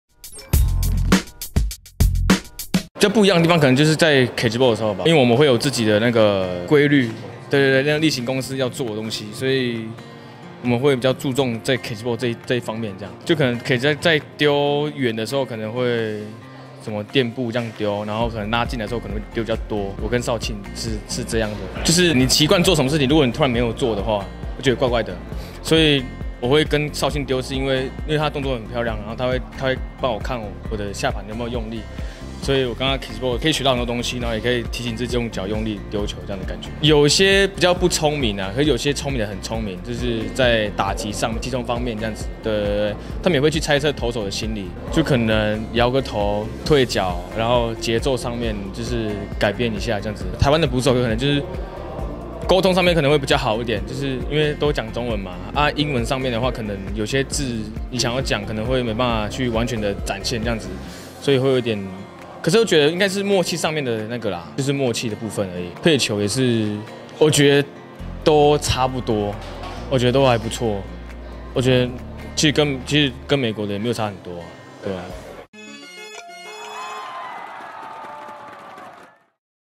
傳球熱身做確實 林凱威小細節不馬虎
簡單卻不簡單！「KGB」看似簡單的熱身方式，林凱威可一點都不馬虎～～～

★TSNA Instagram 在這兒 Follow起來!